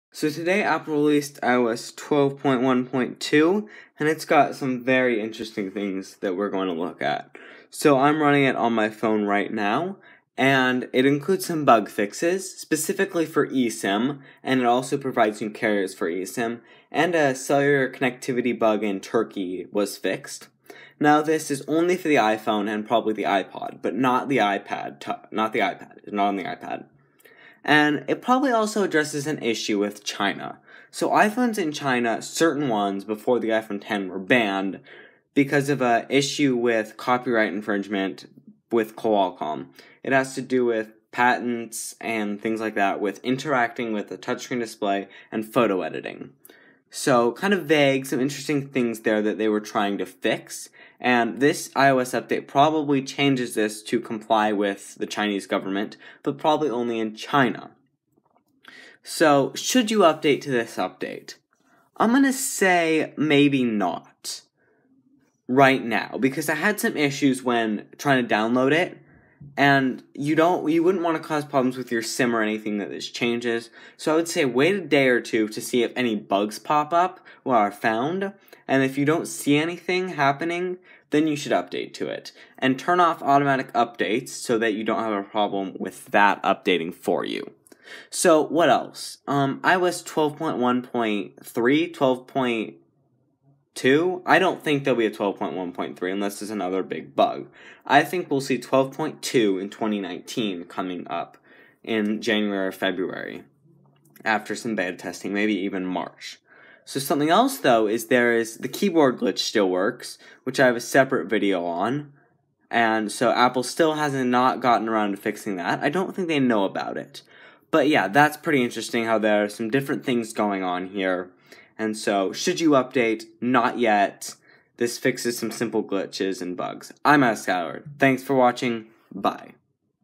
iOS 12.1.2 Released | Update? Bugs?
Don’t update unless you need to fix cellular issues, there are a few new bugs.

Today Apple released iOS 12.1.2, but does it have more bugs or fix the current ones?